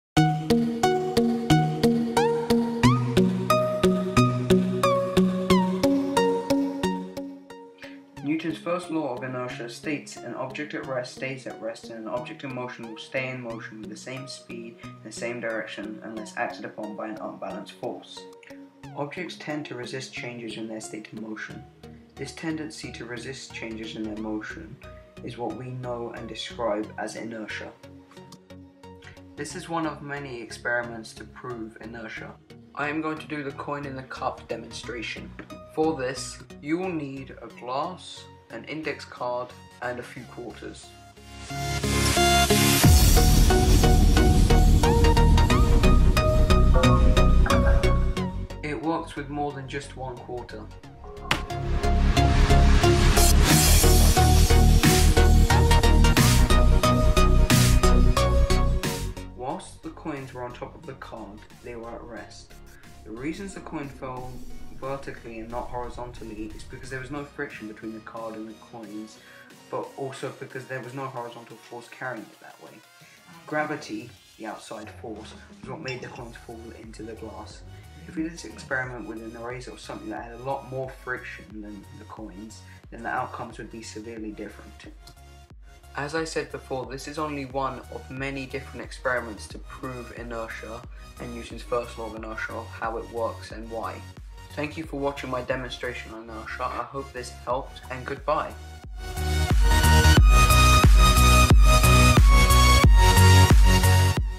Newtons 1st Law of Inertia (Coin in cup)
Here is a visual and physical demonstration of Newton's first law of inertia